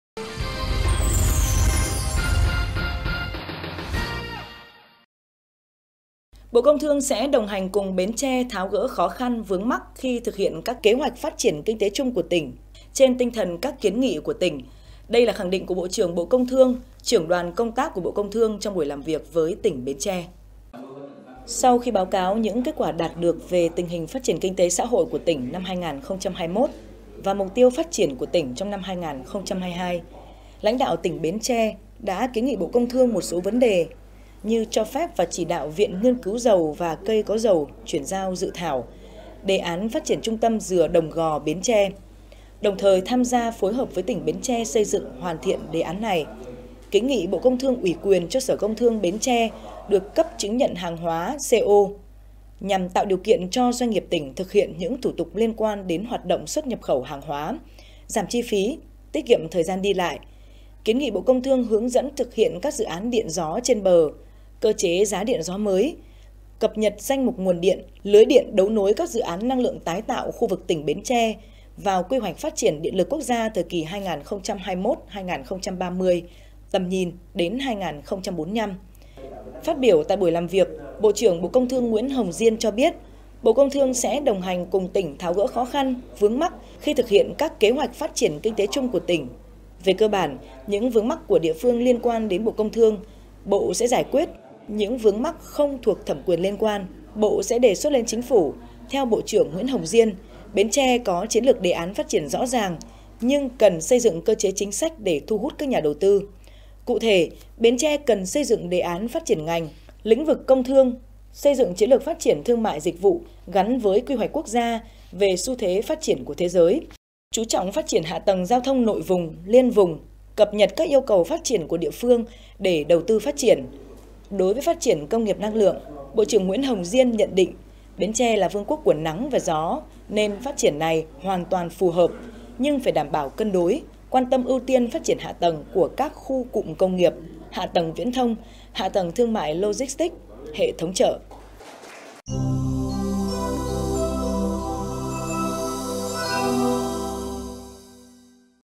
Bộ Công Thương đồng hành cùng Bến Tre phát triển Công nghiệp - Thương mại - Năng lượng
Bộ Công Thương sẽ đồng hành cùng Bến Tre tháo gỡ khó khăn, vướng mắc khi thực hiện các kế hoạch phát triển kinh tế chung của tỉnh trên tinh thần các kiến nghị của tỉnh. Đây là khẳng định của Bộ trưởng Bộ Công Thương - Trưởng đoàn công tác của Bộ Công Thương trong buổi làm việc với tỉnh Bến Tre…